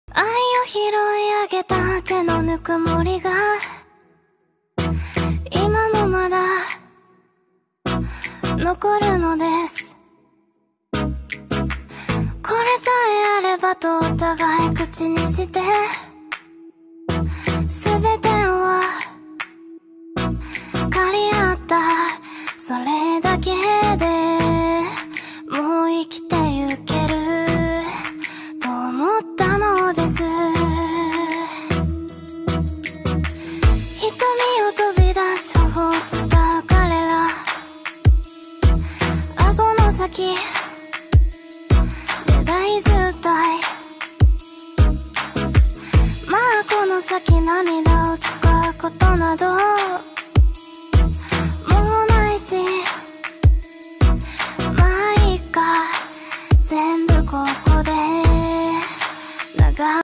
Simple Edit Trade || with Kochii Isekai
I need content, So...I'm doing Edit Trading with @Kochii Isekai (I can't tag you, Sorry)